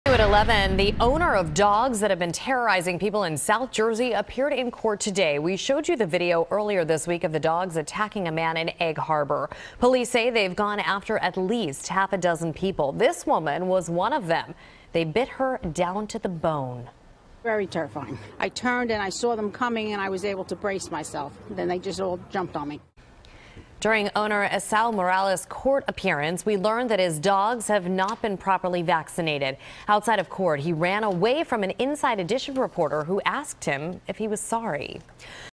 Owner Of Dogs That Have Been Terrorizing People In South Jersey Appears In Court
Police say the dogs have gone after at least half a dozen people.